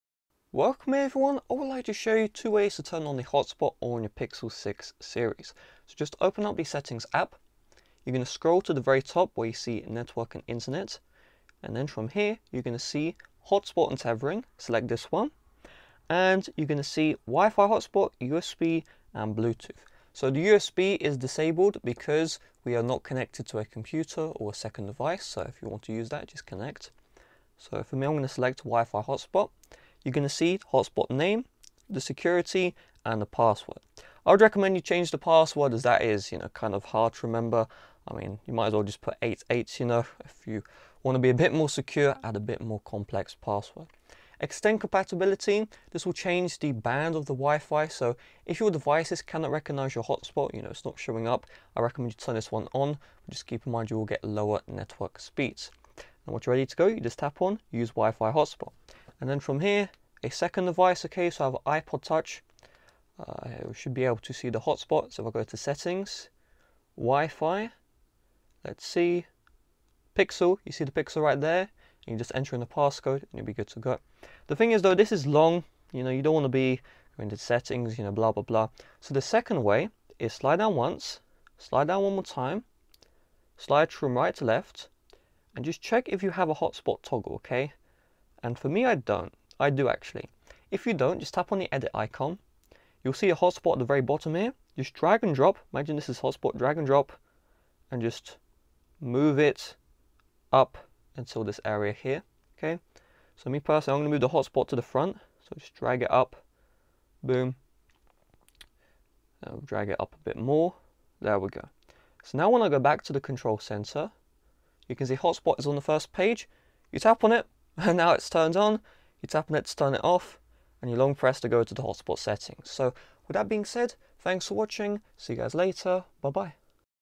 How To Turn On Hotspot on Google Pixel 6 / 6 Pro [2 ways]
Google Pixel 6 enable / disable hotspot tethering | How to turn on hotspot on Google Pixel 6 or 6 Pro! Do you own the Google Pixel 6 Series and want to turn on hotspot? If so, this video is for you! I will teach you how to turn on hotspot on Google Pixel 6 or 6 Pro in this tutorial. By the end of this guide, you will know 2 ways to turn on and setup hotspot on the Google Pixel 6 or 6 Pro. You will also know how to use WiFi, BlueTooth or USB tethering as well.